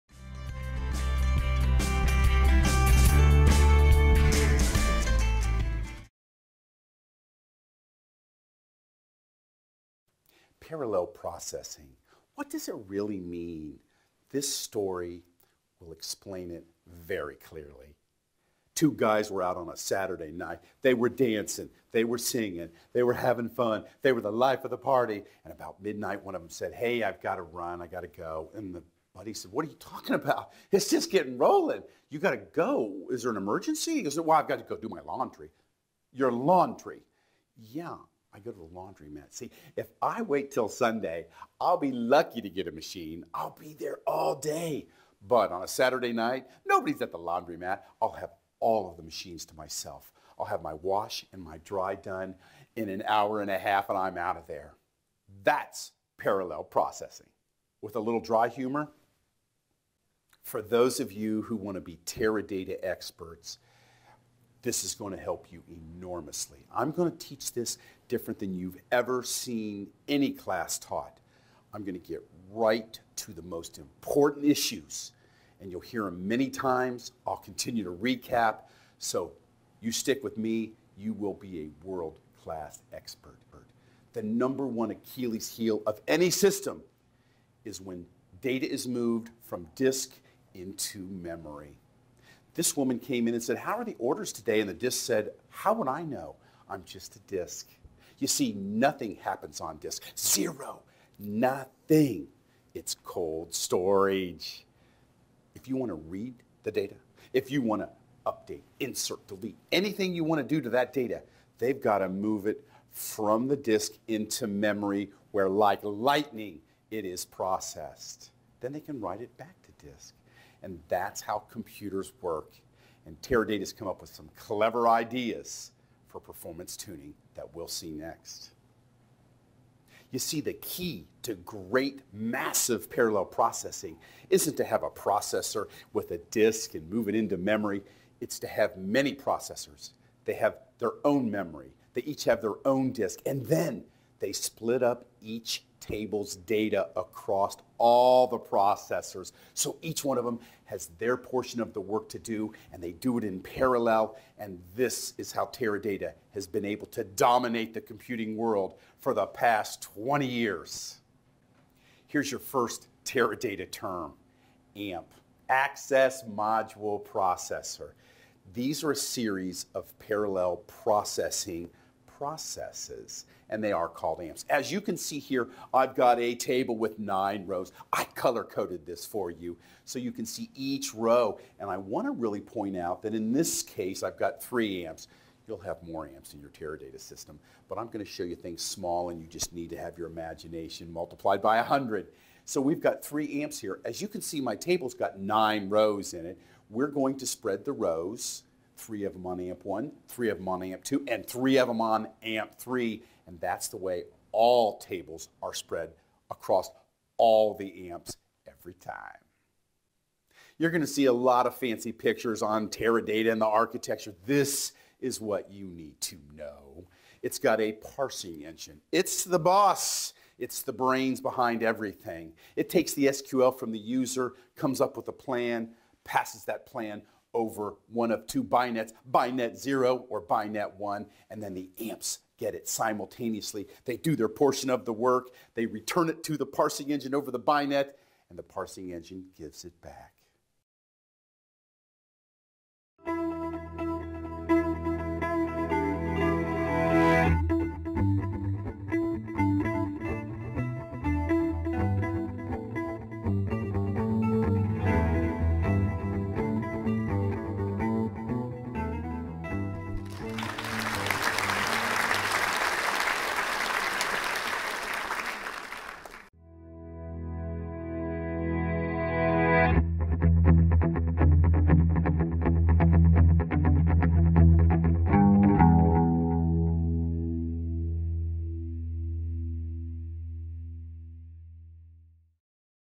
Let's Learn Teradata Lesson 1: Parallel Processing
Explore Teradata with Tera-Tom of Coffing Data Warehousing! In this lesson, get a clearer understanding of what  parallel processing is.